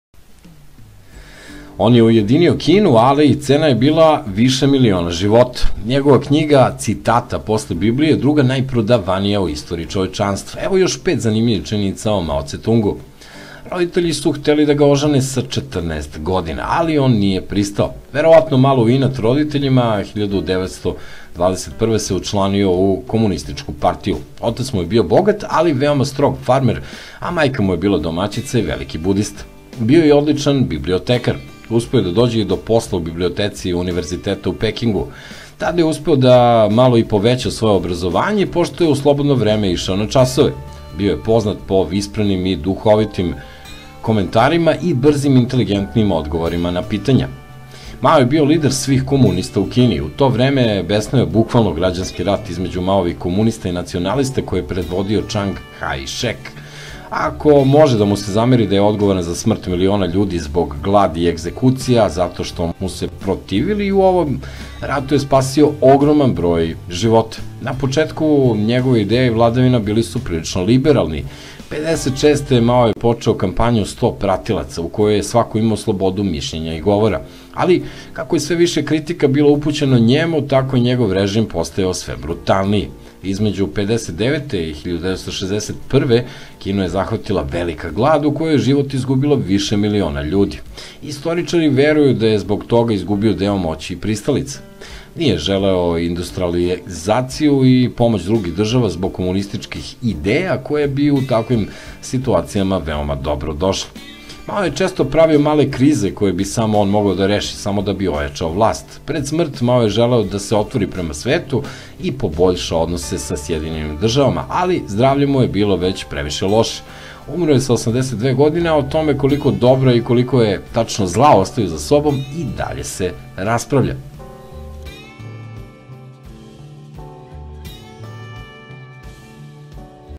Mao Cetung, Otac i Uništitelj Kine
Mao Cetung je bio vođa kineskih komunista. Njegovi citati su druga najprodavanija na svetu posle biblije. Pod njegovim vođstvom, umrlo je više miliona kineza, a svako ko mu se protivio, bio je brutalno kažnjavan. Ipak, mnogi stanovnici te zemlje ga i dalje vide kao Idola